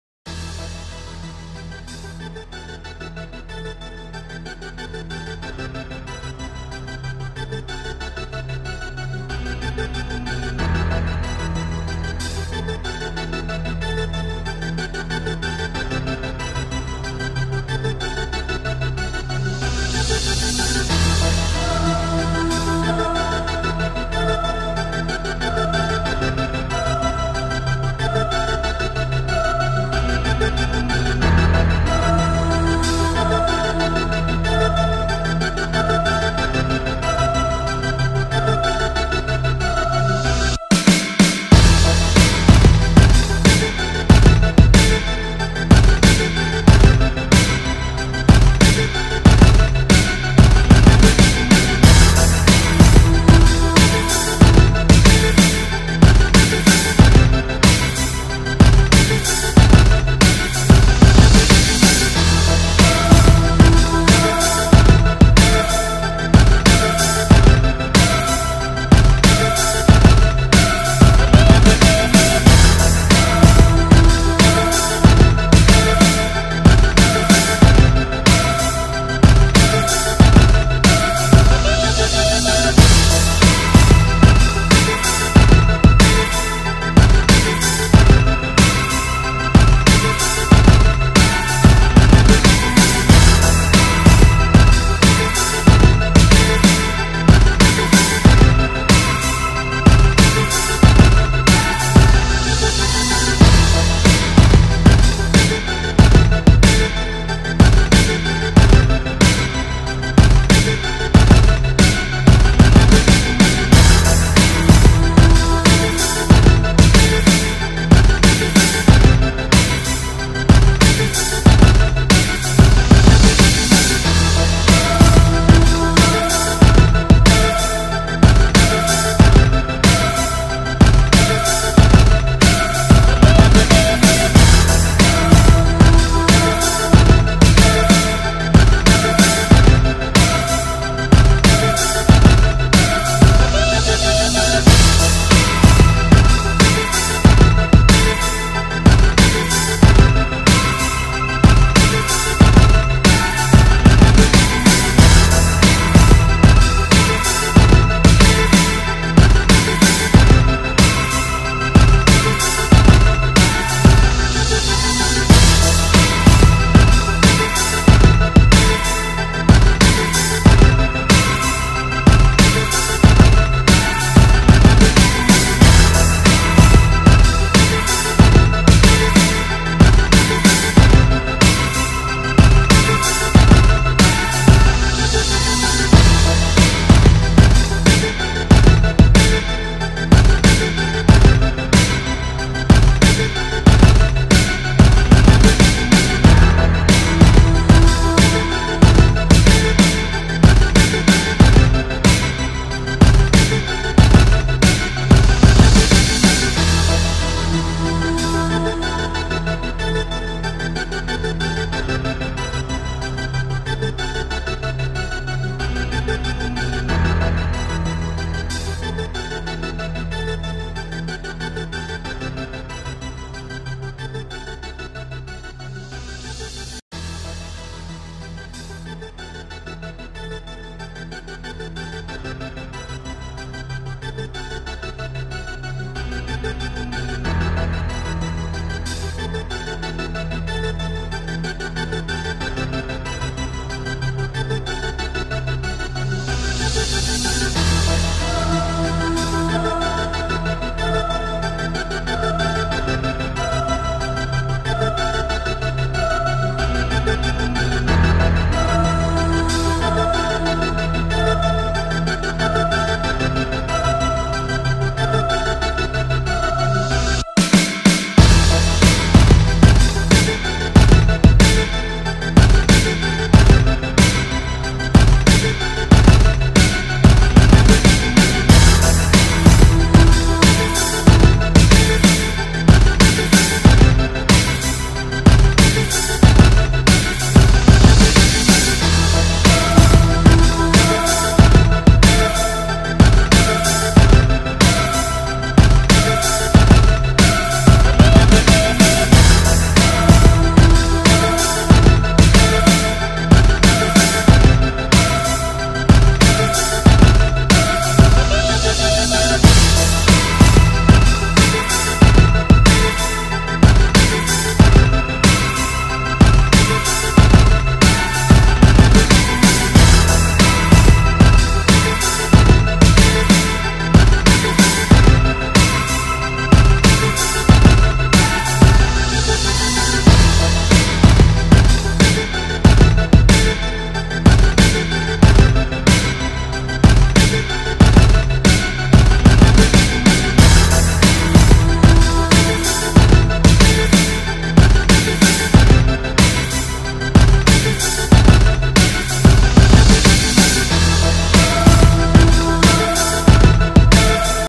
ARM WRESTLING TRAINING 2019 #3
This was a pretty successful training session as my elbows are finally completely recovered from an injury which happened to me about 2 months ago(sprained elbow tendons).